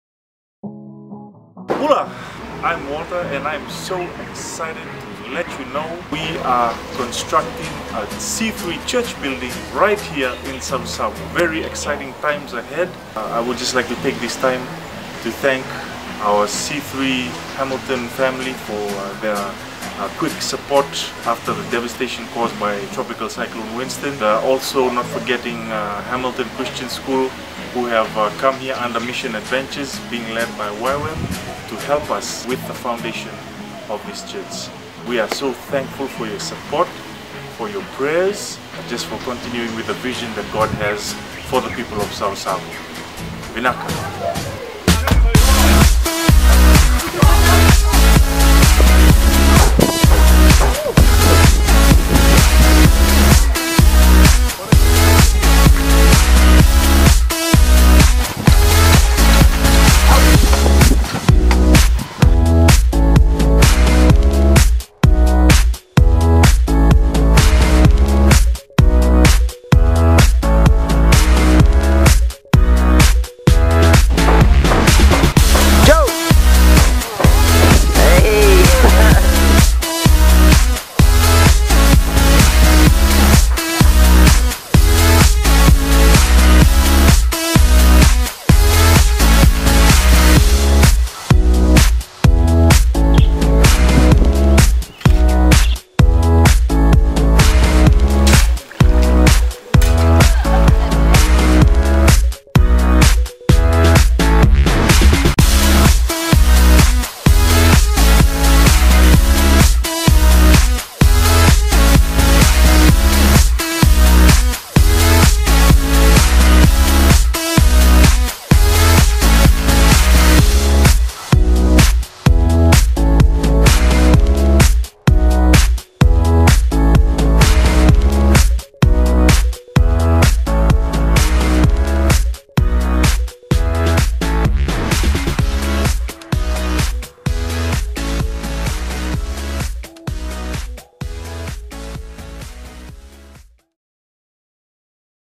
C3 SAVUSAVU Church Build Phase 1
We are nearing completion of the concrete foundation of our c3 church building here in Savusavu.